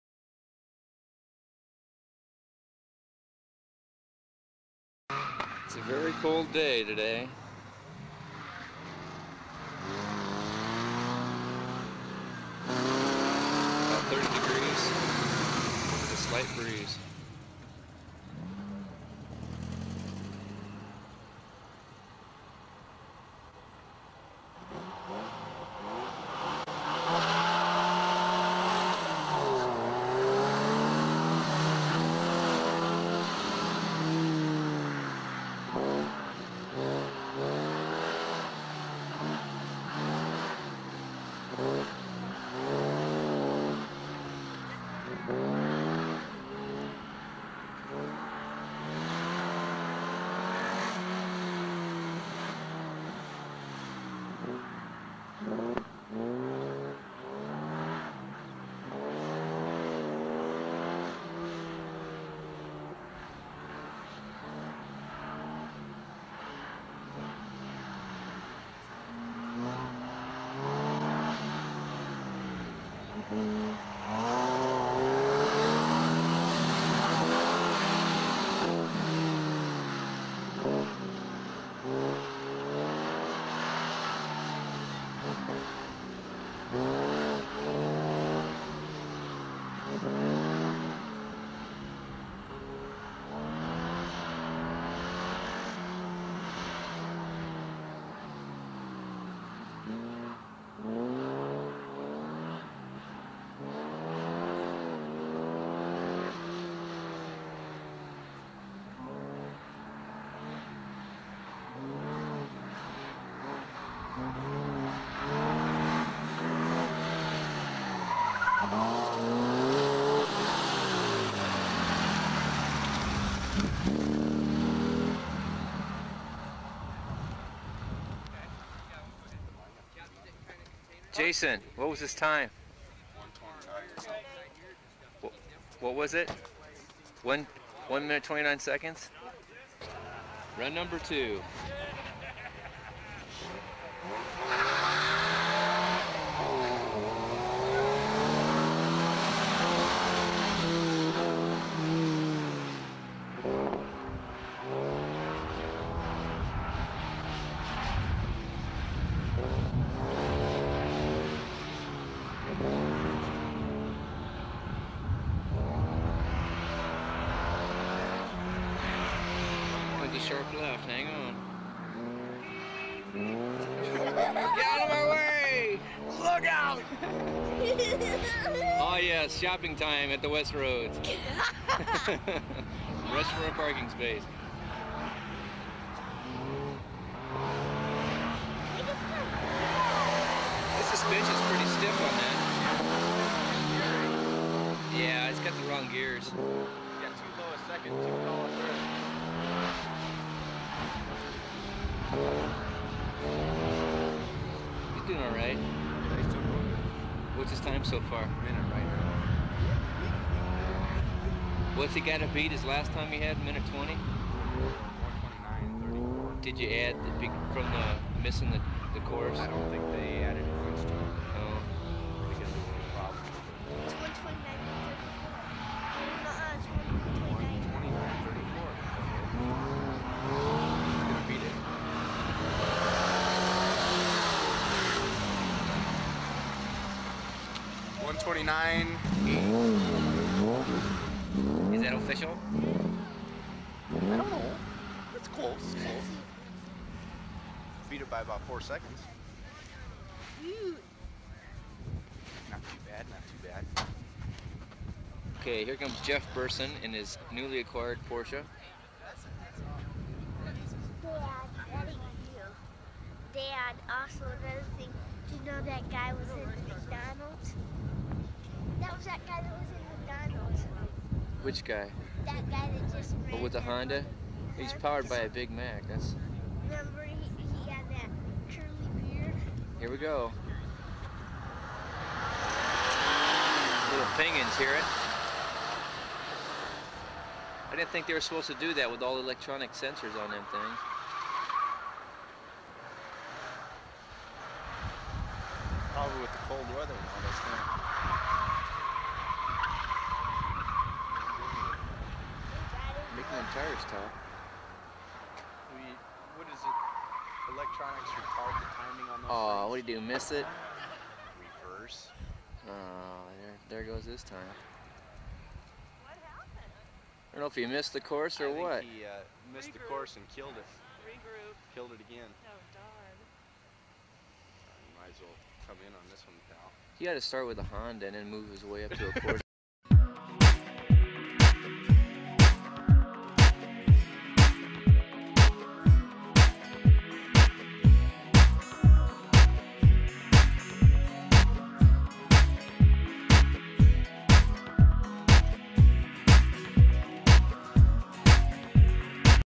Ken Burson Collection Video 4 - NRSCCA Solo, February 15, 1987
Ken Burson Collection Videos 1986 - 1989

Video #4 – NRSCCA Solo February 15, 1987

(Note 1: This is likely a NRSCCA event. Most 1987 EXHAUST NOTES newsletters are missing so there are no records to confirm. NRSCCA member Jerry Doctor and his car are shown at this event. Jerry can be seen in early video images.)

(Note 2: The video is mostly images of Ken’s Saab and of his brother’s Porsche. However, the video shows an autocross venue at Aksarben in Omaha in winter on a decent day for February.)

This is a Nebraska Region Sports Car Club of America (NRSCCA) Solo event at Aksarben, 6800 Mercy Road, Omaha, NE; Lot #4.

The first few frames shows NRSCCA Treasurer and Membership Director, Jerry Doctor. He is the one on the far left of the frame, sun glasses and blue jacket

The following is the list of cars and drivers in order of appearance.

1) The first car shown running the course is a bronze 1969 Saab Sonett V4 driven by Ken Burson competing in ES (E Stock) class. This is Ken’s first run.

2) Ken’s nephew, Jason, was the personal time keeper providing an immediate run time.

3) Ken Burson makes his second run in his 1969 Saab Sonett V4.

4) As the video photographer follows Ken to his parking location, Jerry Doctor’s light blue early to mid-80’s Fiat Pininfarina comes into view in the parking area.

5) Jeff Burson in his newly acquired Porsche is next. Unfortunately, half way through his run Jeff takes a wide turn where he would miss a gate. This caused him to stall the car out and that ended the video.

The NRSCCA EXHAUST NOTES newsletter archives for January – April 1987 are missing so there are no results available of this event.

Lastly is a 1993 aerial photograph of Aksarben showing the location and size of Lot #4. This lot is typical of so many other solo event locations in Omaha that autocrossers competed at previously. The Aksarben (Nebraska spelled backwards) Race Track and Coliseum was an indoor arena and horse racing complex located in Omaha, Nebraska. Built to fund the civic and philanthropic activities of the Knights of Aksarben, the thoroughbred race track was built in 1919, and the Coliseum in 1929. The racetrack closed in 1995 and the arena in 2002. The facility was demolished in 2005, and is currently being redeveloped for a variety of uses, including dormitory housing for the University of Nebraska at Omaha and the Aksarben Village development.